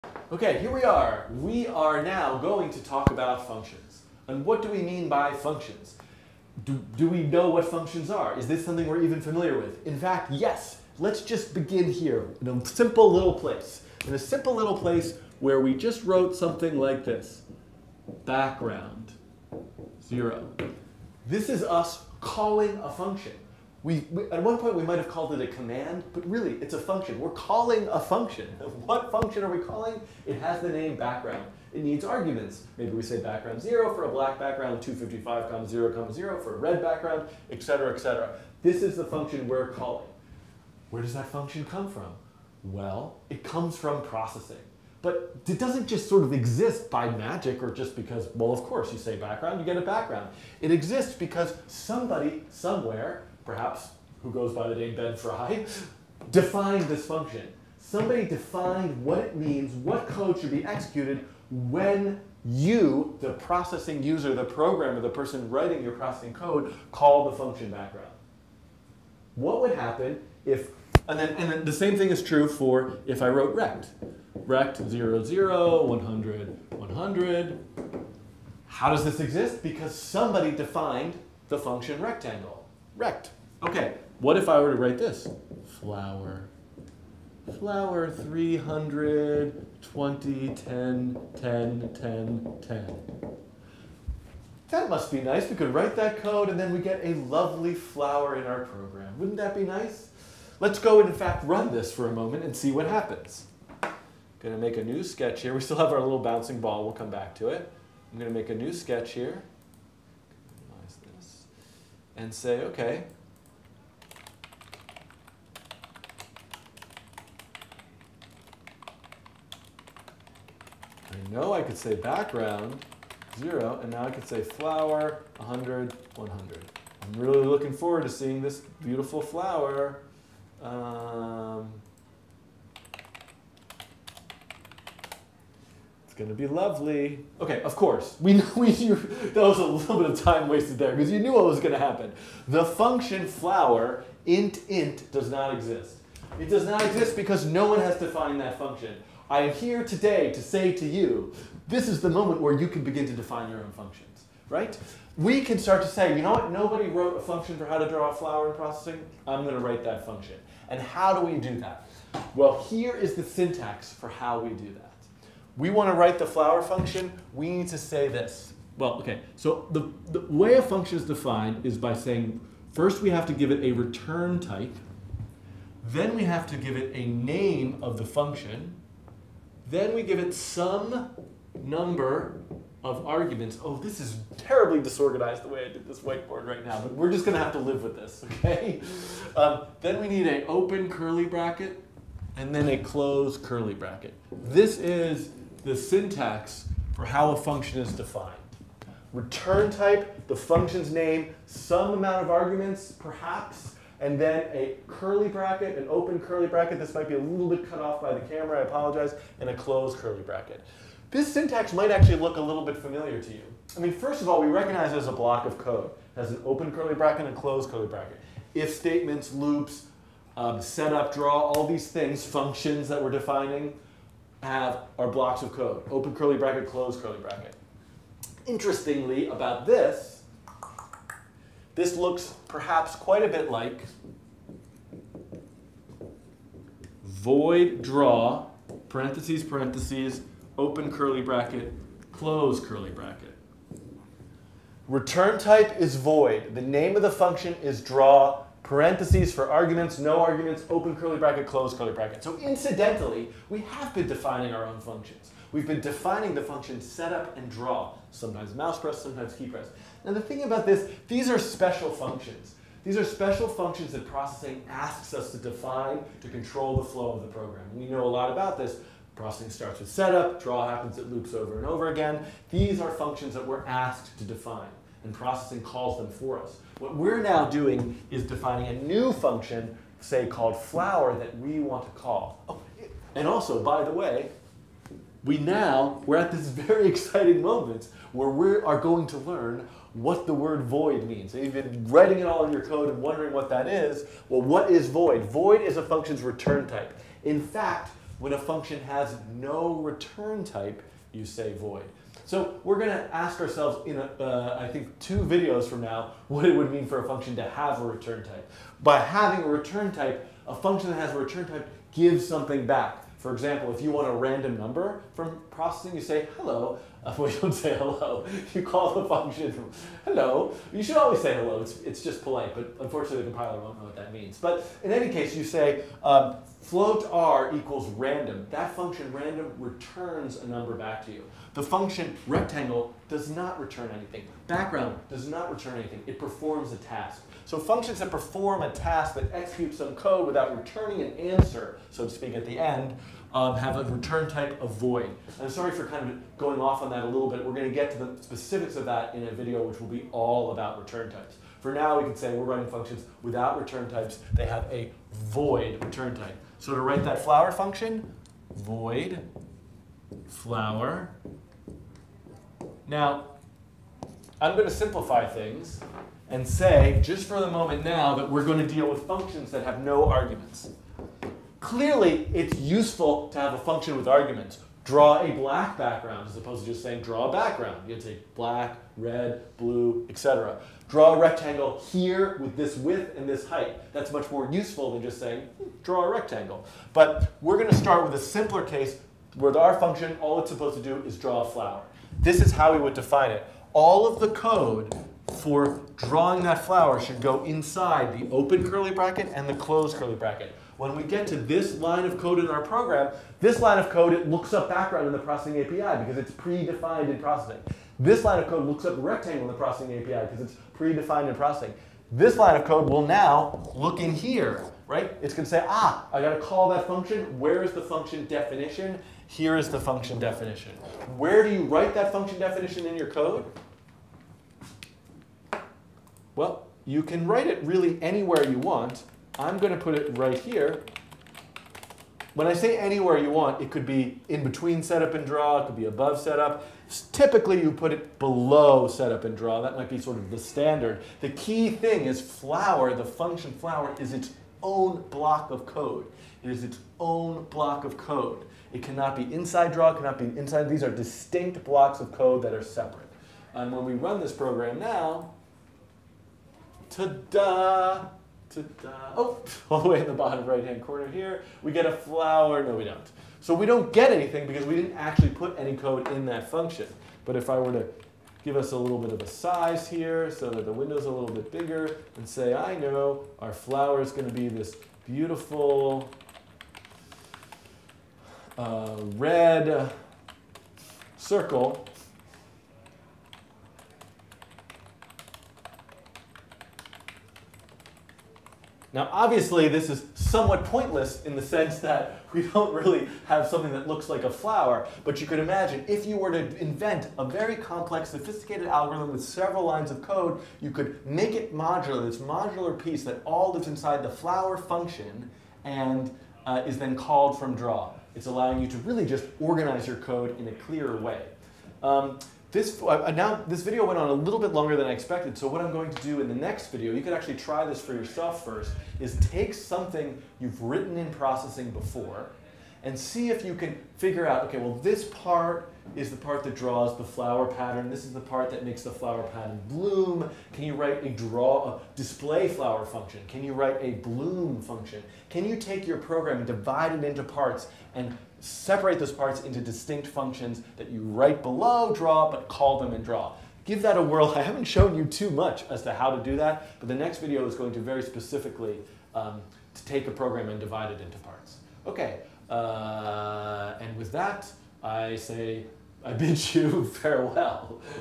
7.2: Functions Basics - Processing Tutorial
This video covers the basics of defining and calling functions in Processing (Java).

It accompanies Chapter 7 of Learning Processing: A Beginner's Guide to Programming Images, Animation, and Interaction

(Note this video was shot in Fall 2012)